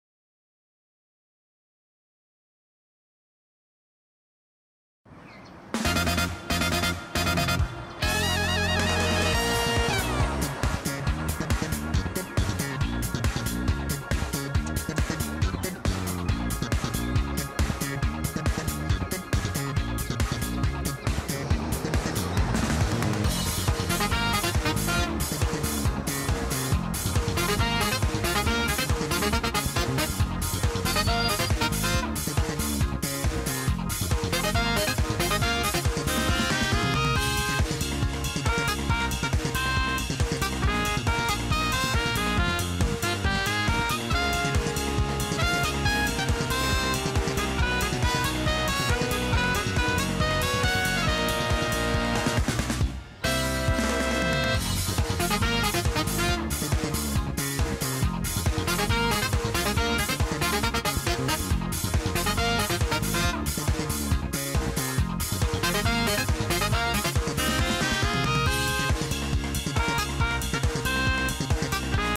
とさでん交通 5月3日は「電車の日」
本日、とさでん交通本社で「電車の日」開催です！